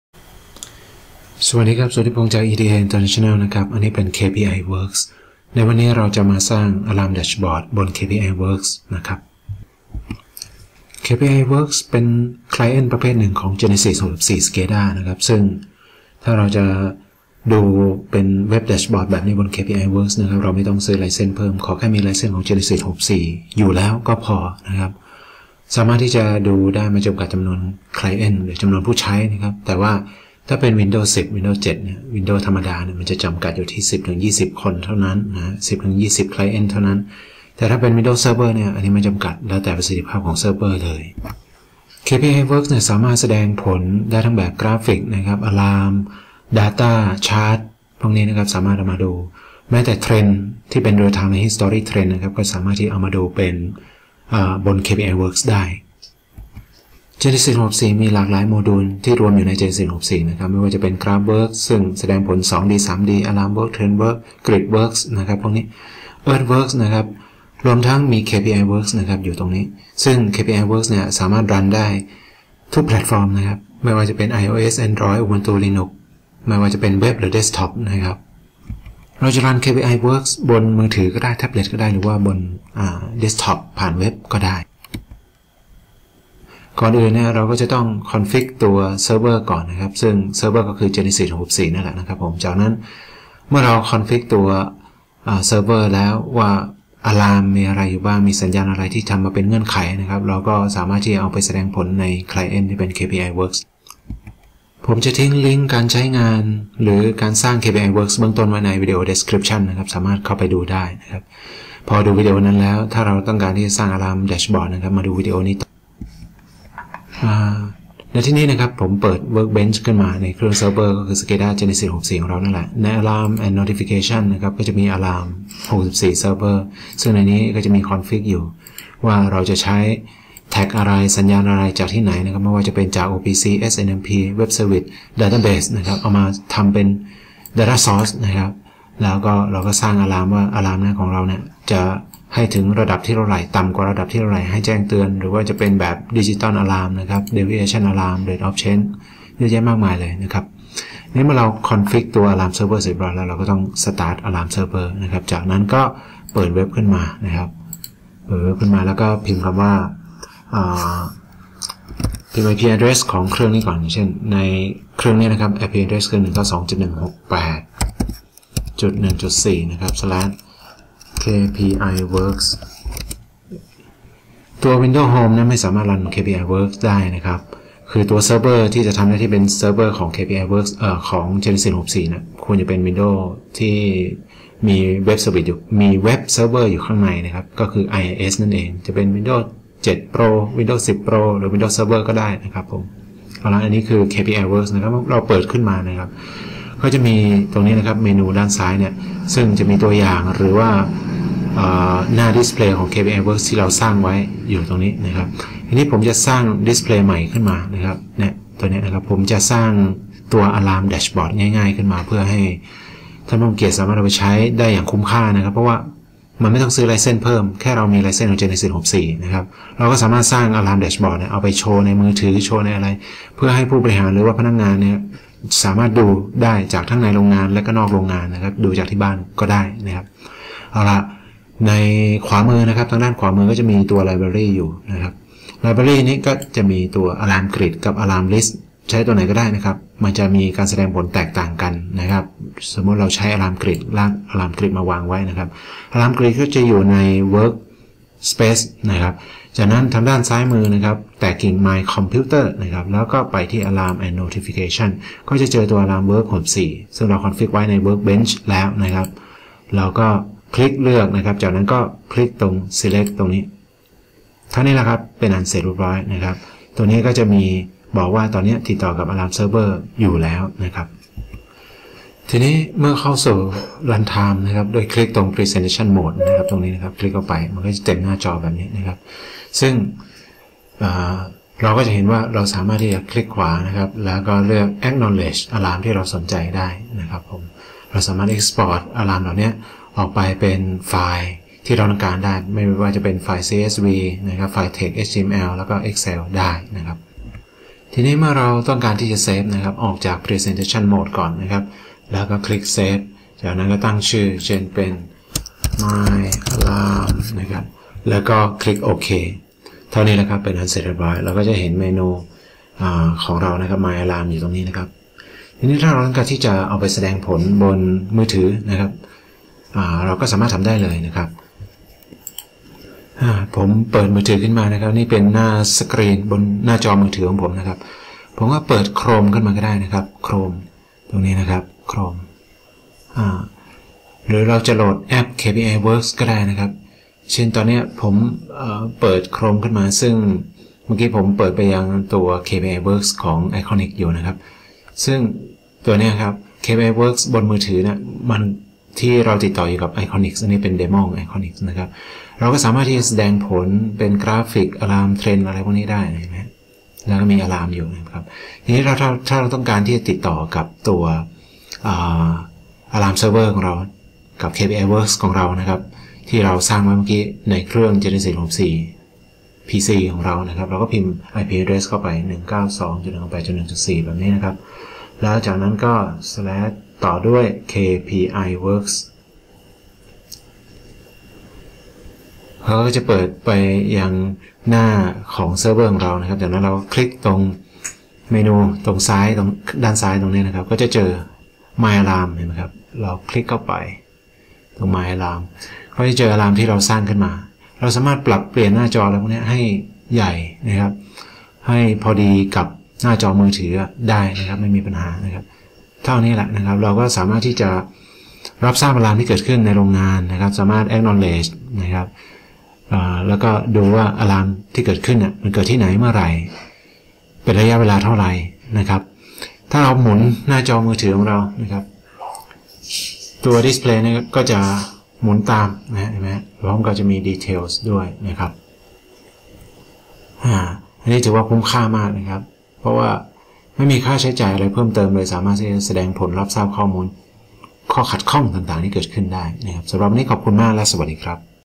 ดูAlarmผ่านมือถือ ไม่จำกัดจำนวน
รับทราบAlarmจากโรงงาน ทุกที่ ทุกเวลา ดูได้ทั้งมือถือ แท็ปเล็ต เว็บ คอมพิวเตอร์ แค่มีGENESIS64

การใช้งานKPIWorXเบื้องต้น